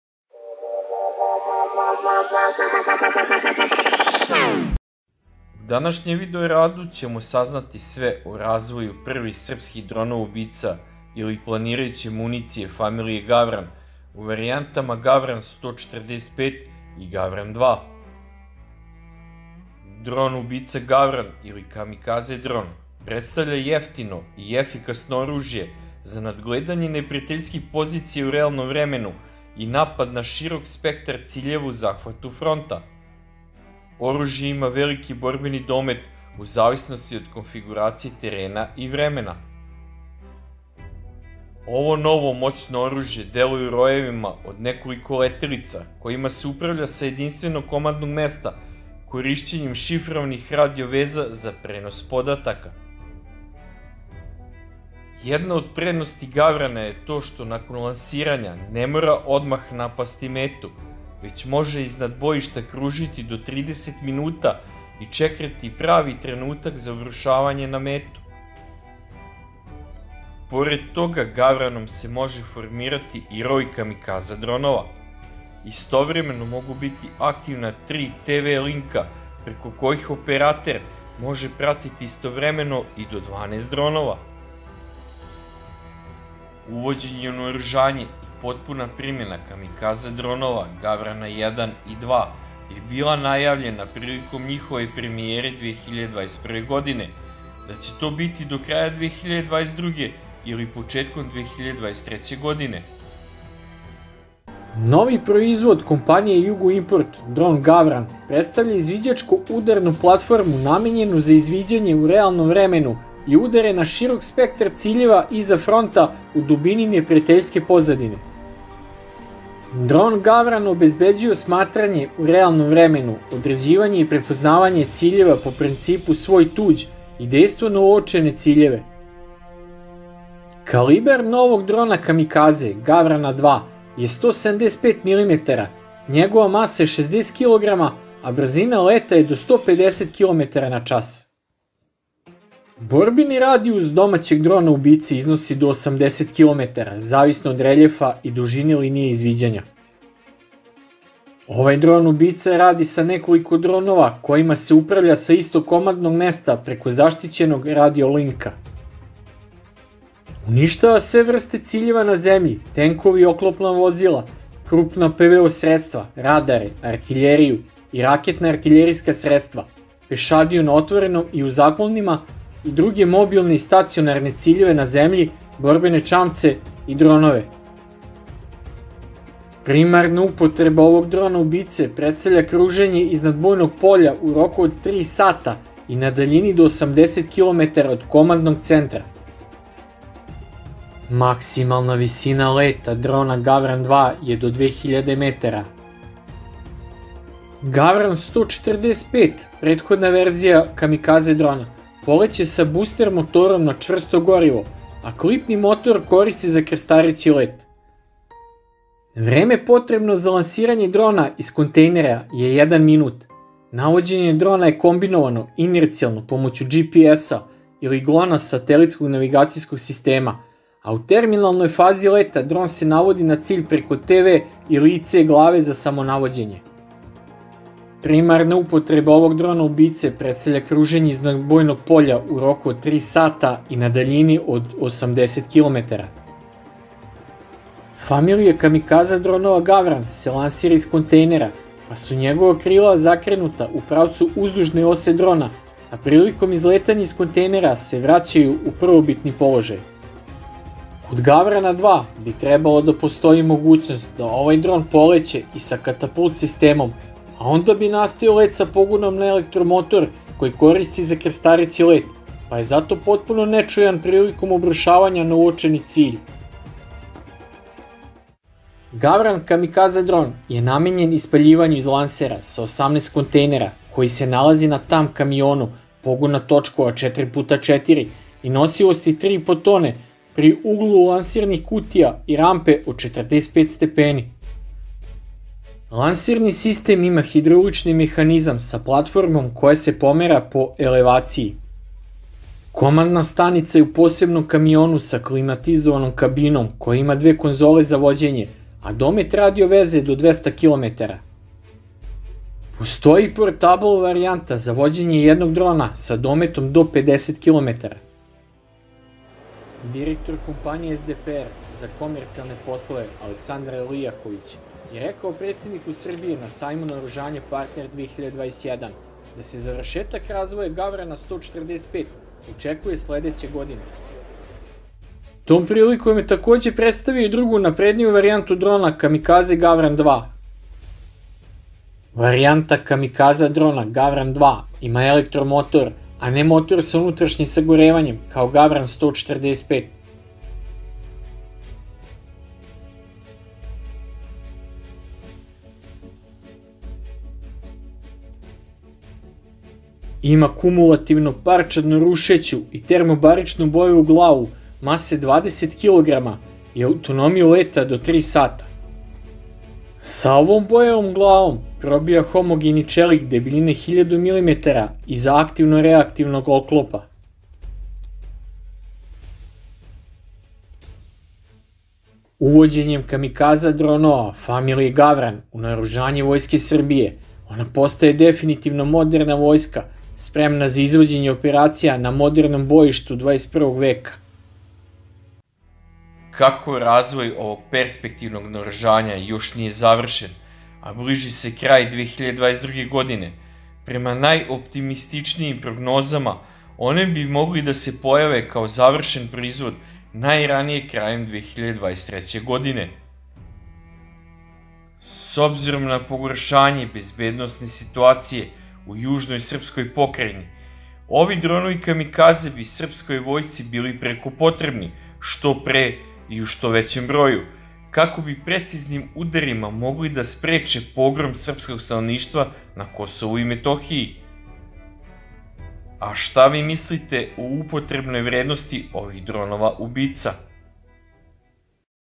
Novi srpski dronovi ubice Gavran. Dokle se stiglo sa razvojem? Serbian kamikaze drones Raven
Everything about the development of the first Serbian killer drones or planning ammunition of the "Gavran" family in the "Gavran 145" and "Gavran 2" variants. The introduction into armament and the full application of kamikaze drones "Raven 1" and "2" was announced during their premiere in 2021, that it would be by the end of 2022 or the beginning of 2023. The "Raven" killer drone or kamikaze drone is a cheap and effective weapon for monitoring enemy positions in real time and attacking a wide range of front-line targets. One of the advantages of the "Raven" is that after launch, it does not have to attack the target immediately, but can circle above the battlefield for up to 30 minutes and wait for the right moment to attack the target.
In addition, the "Raven" can also form a swarm of kamikaze drones. 3 TV links can be active at the same time, through which the operator can monitor up to 12 drones at the same time.
Sve o razvoju prvih srpski dronova ubica ili planirajuće municije familije “Gavran” u varijantama “Gavran 145” i “Gavran 2”. Uvođenje u naoružanje i potpuna primena kamikaza dronova "Gavrana 1" i "2" je bila najavljena prilikom njihove premijere 2021. godine da će biti do kraja 2022 ili početkom 2023 godine. Dron ubica "Gavran" ili kamikaze dron predstavlja jeftino i efikasno oružje za nadgledanje neprijateljskih pozicija u realnom vremenu i napad na širok spektar ciljeva u zahvatu fronta. Jedna od prednosti "Gavrana" je ta što nakon lansiranja, ne mora odmah napasti metu, već može iznad bojišta kružiti do 30 minuta i čekati pravi trenutak za obrušavanje na metu.
Pored toga, "Gavranom" se može formirati i roj kamikaza dronova. Istovremeno mogu biti aktivna 3 TV linka preko kojih operater može pratiti istovremeno i do 12 dronova.